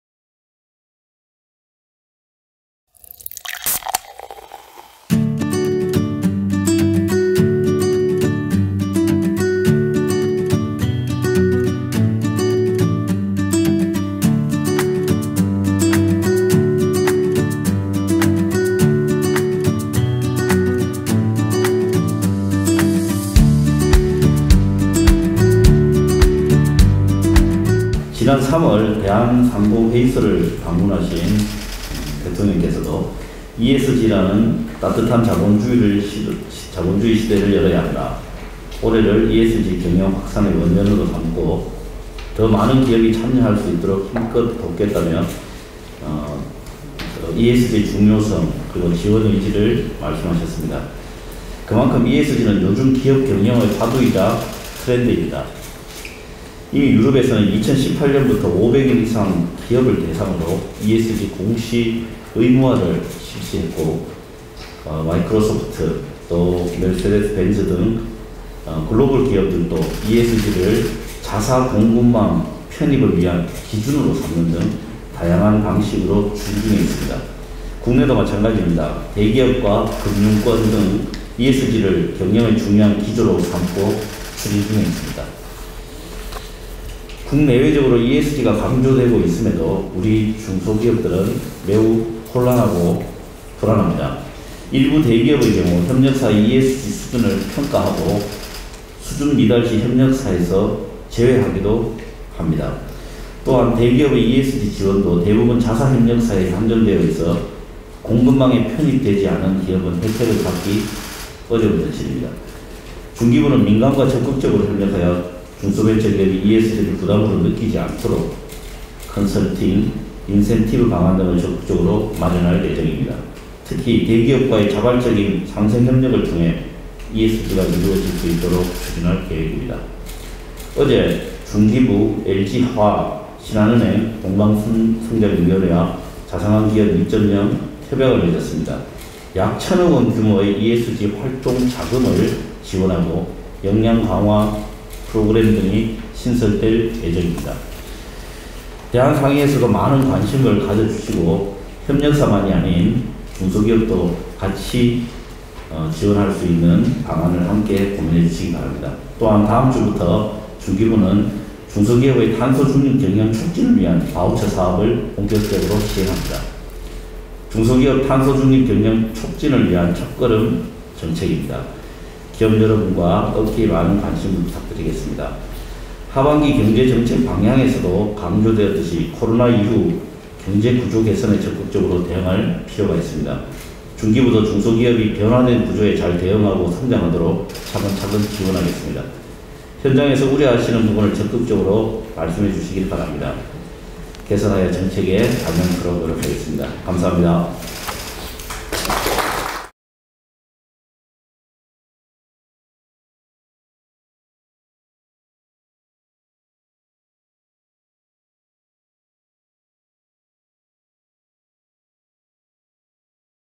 중소벤처기업 ESG 간담회 [속이 뻥! 현장 칠승사이다]
7월 8일(목), 서울 대한상공회의소 체임버 라운지에서
환경·사회·지배구조(ESG) 경영에 관심을 갖고 있는
중소기업들과 간담회를 가졌다

"국내외적으로 ESG 강조되고 있음에도
우리 중소기업들은 매우 혼란하고 불안합니다

중기부는 민간과 적극적으로 협력하여
중소벤처기업이 ESG를
부담으로 느끼지 않도록 컨설팅,
인센티브 방안 등을
적극적으로 마련할 예정입니다

하반기 경제정책 방향에서도
강조되었듯이
코로나 이후 경제 구조 개선에
적극적으로 대응할 필요가 있습니다

중기부도 중소기업이 변화된 구조에
잘 대응하고 성장하도록
차근차근 지원하겠습니다"

- 중기부 권칠승 장관 -